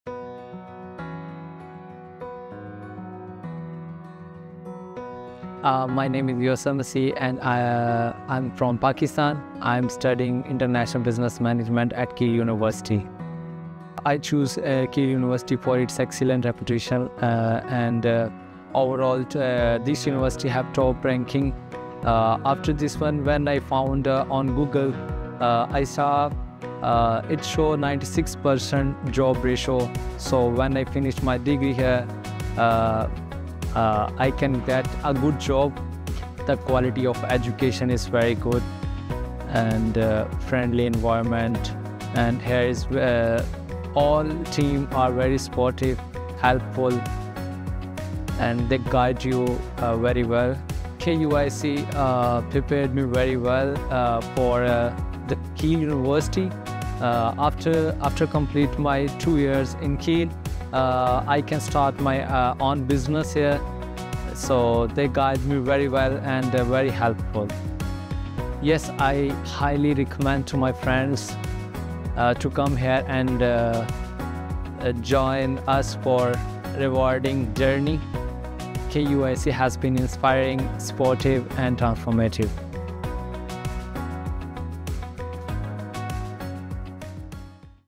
The KUIC Experience - Yousaf Masih
Yousaf, from Pakistan, has progressed from KUIC to Keele University to study International Business Management. Hear about his experience and why he would recommend KUIC to others!